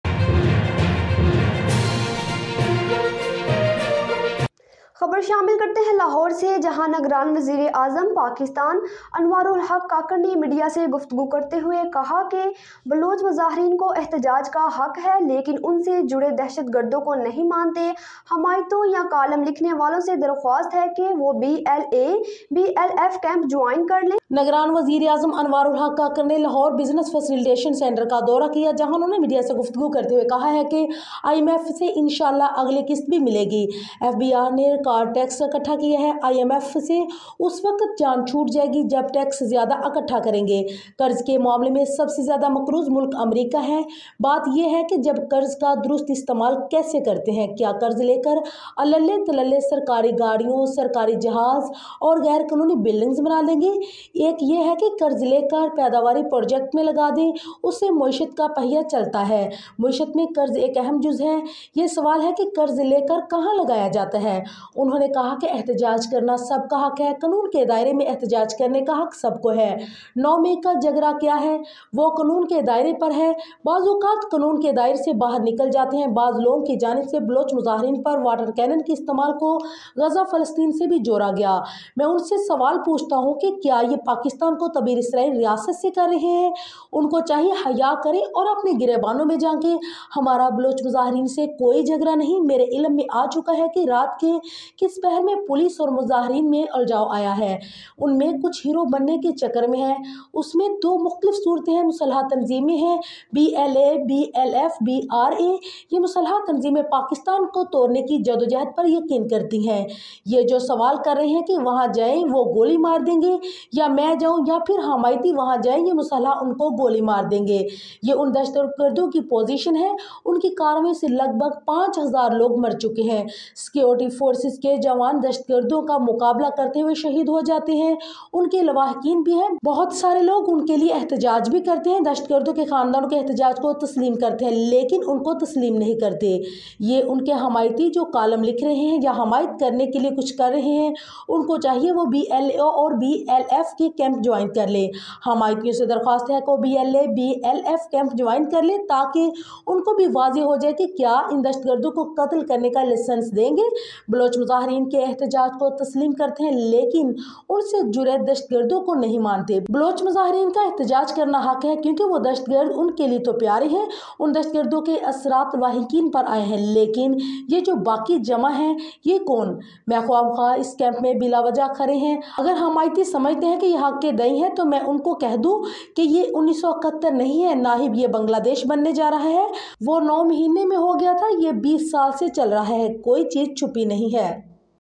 care taker Anwaar Ul Haq big statement | بلوچ مظاہرین کو احتجاج کا حق ہے | n3hd | sufia malik
بلوچ مظاہرین کو احتجاج کا حق ہے لیکن ان سے جڑے دہشتگردوں کو نہیں مانتے
حمایتیوں یا کالم لکھنے والوں سے درخواست ہے کہ وہ بی ایل اے، بی ایل ایف کیمپ جوائن کرلیں، تاکہ ان کا بھی واضح ہو جائے، کیا ہم ان دہشتگردوں کو قتل کرنے کا لائسنس دے دیں؟ نگران وزیراعظم انوارالحق کاکڑ کا سوال